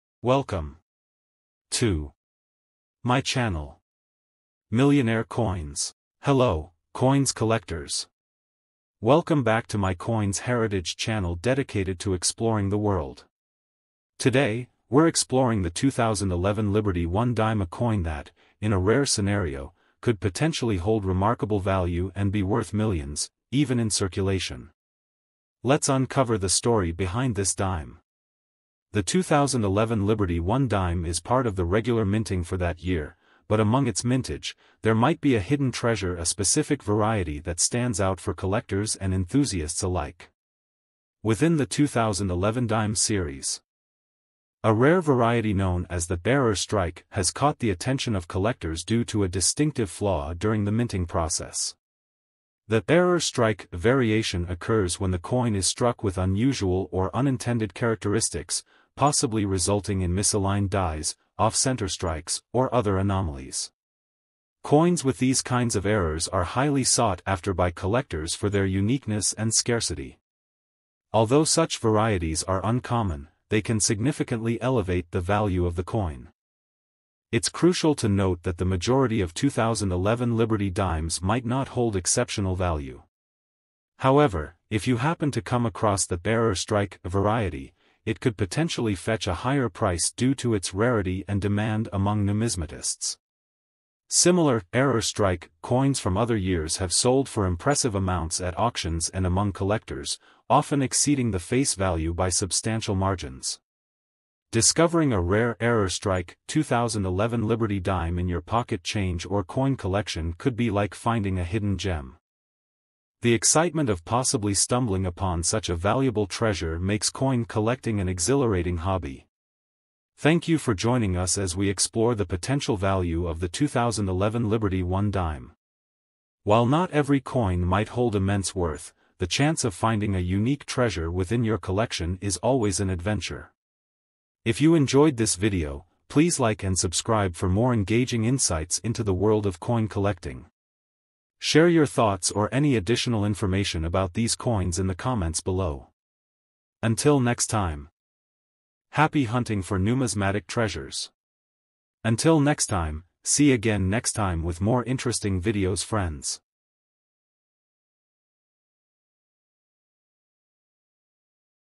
Unveiling the Potential Value of the 2011 USA One Dime Coin
Welcome, coin enthusiasts, to an exciting exploration into the world of numismatics! Join us as we delve into the potential worth of the 2011 USA One Dime coin, a seemingly ordinary piece of currency that could hold surprising value.

In this video, we meticulously examine the intricacies of the 2011 USA dime, exploring its design features, possible minting variations, and factors that could elevate its value beyond its face denomination. While often disregarded, certain editions or specific characteristics of this dime might make it a coveted find for collectors and enthusiasts alike.

Throughout our investigation, we'll spotlight distinctive attributes to watch for, including mint marks, potential die varieties, or any known errors from the production process that might significantly increase the coin's desirability among collectors. Our aim is to shed light on why this particular dime from 2011 might possess more value than anticipated.

Whether you're a seasoned collector with a keen eye for rare finds, an amateur numismatist eager to learn about valuable coins, or simply someone intrigued by the hidden treasures within your pocket change, this video strives to uncover the extraordinary aspects of the 2011 USA One Dime coin that could elevate it to a prized addition to your collection.

Don't miss out on this opportunity to uncover a potential hidden gem!